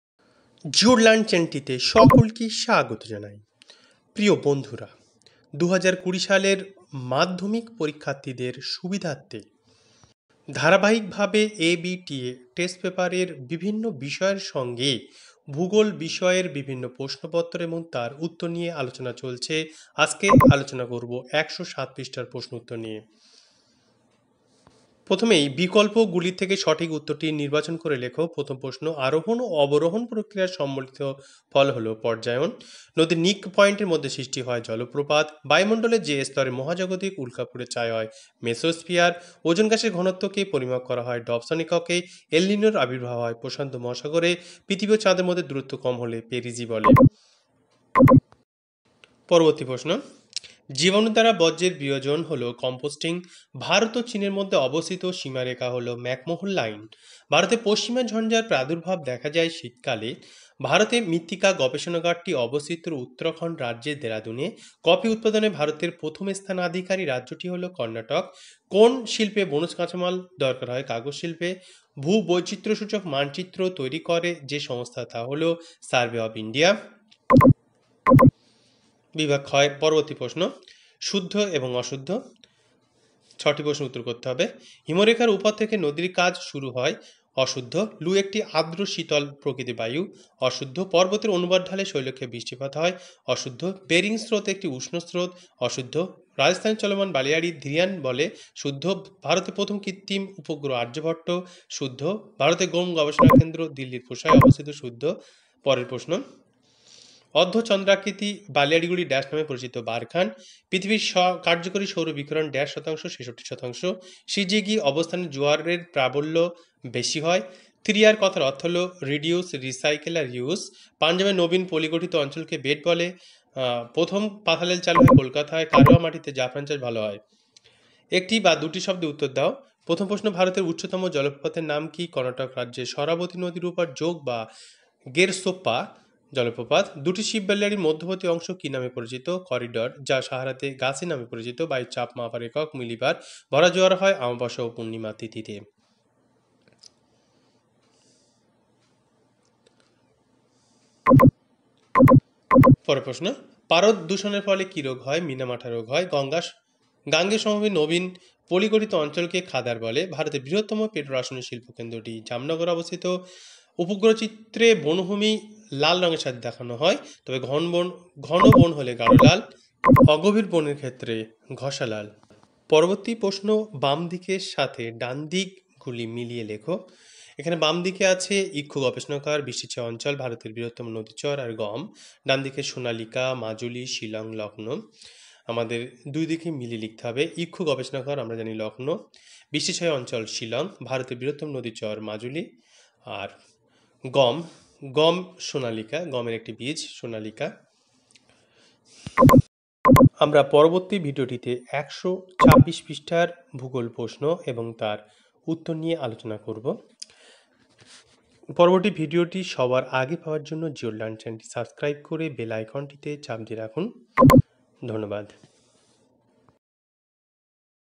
Page No - 107.Madhyamik Test Paper Solved /2020/ ABTA Test Paper Solved / Geography.
ABTA TESTPAPER SOLVED-2020#GEOGRAPHY

নদীপ্রবাহ . আদর্শ নদী কাকে বলে ?  River Course.What is Ideal River ? The Three Courses of a River?